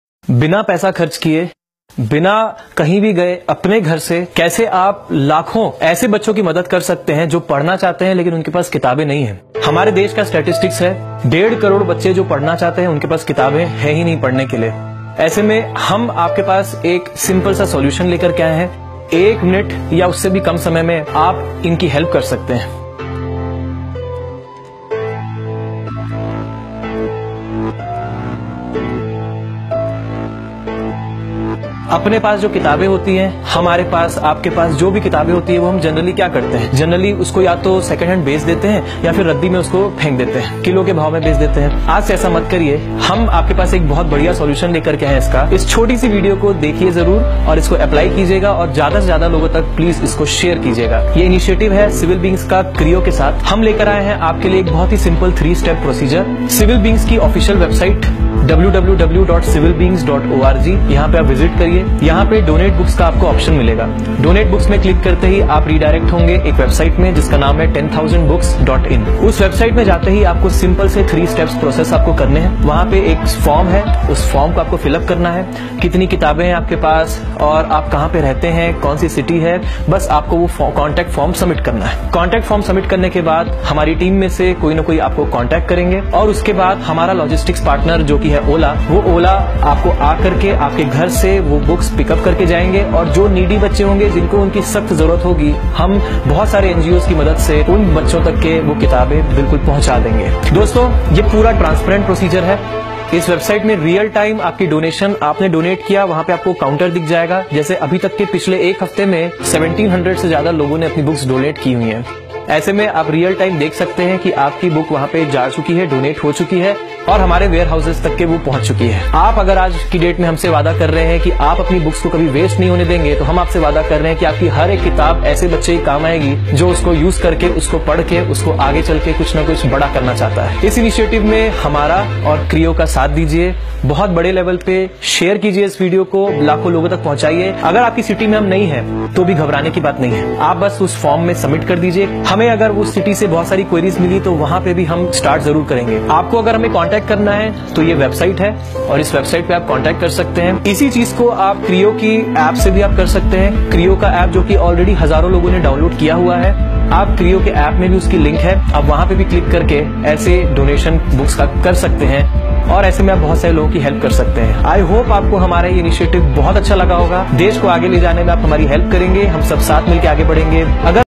SERVICE TO MANKIND SERVICE TO GOD
Jo garib bacha padh nehin sakte hein unko aap book donate kar sakte hein must watch this video